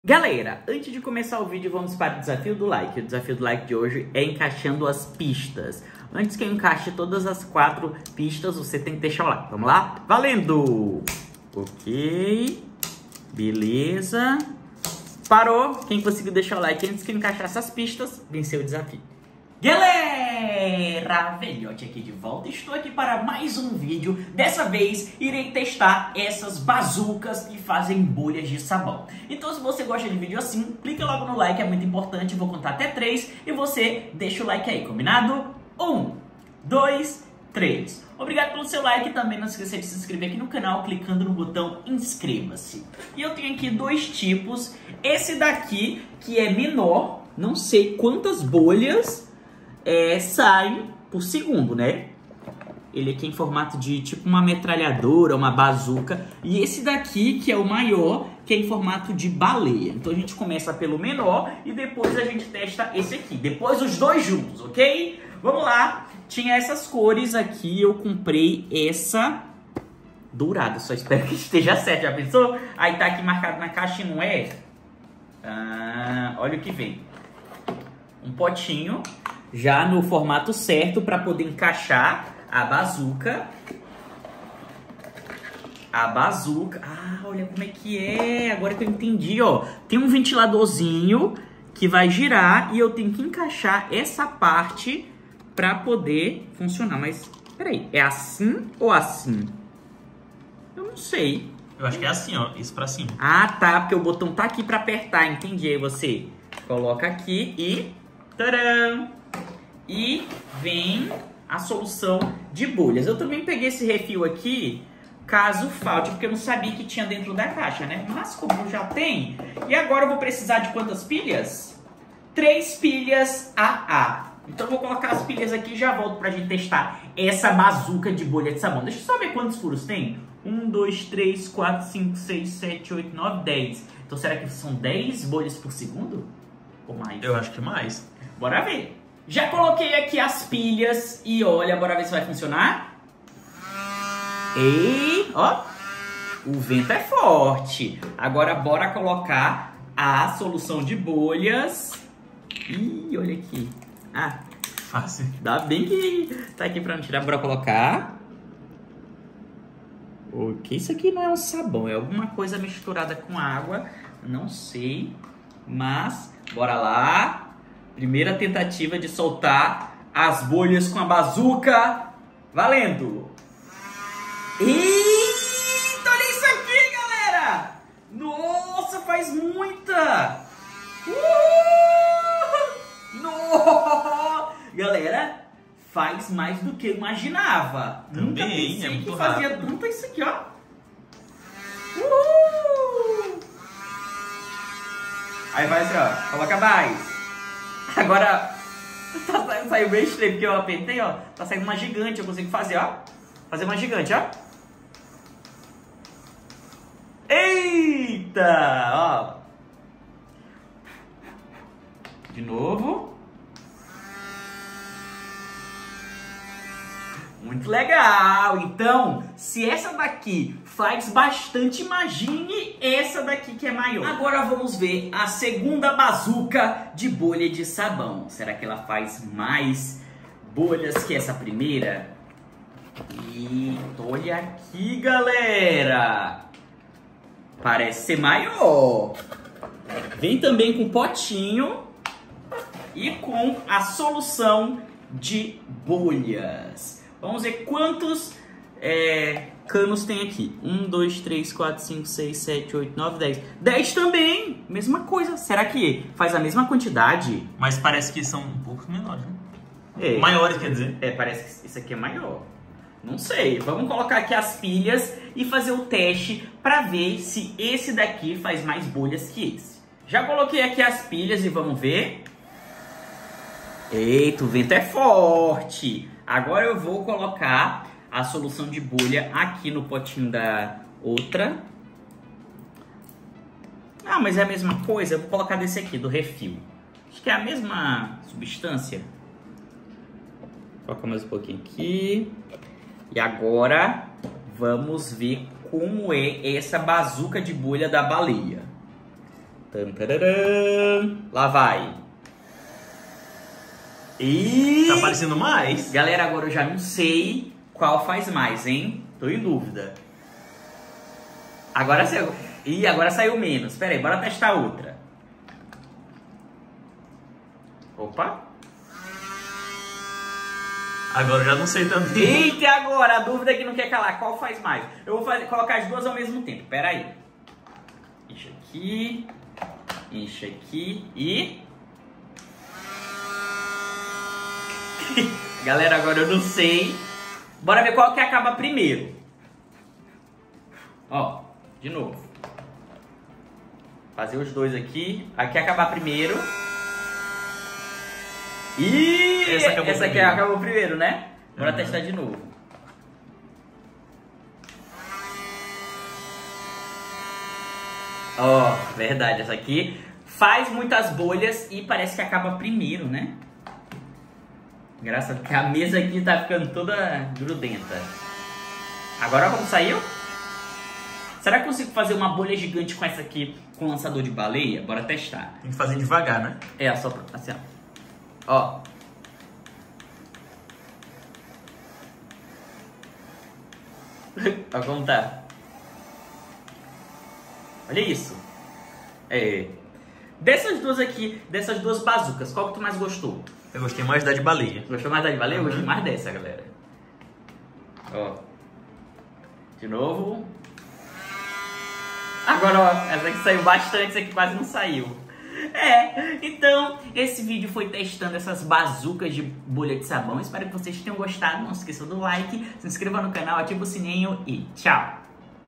TESTANDO BAZUCAS QUE LANÇAM BOLHAS DE SABÃO!!!
Nesse vídeo o Velhote testou 2 bazucas de bolhas de sabão diferentes!!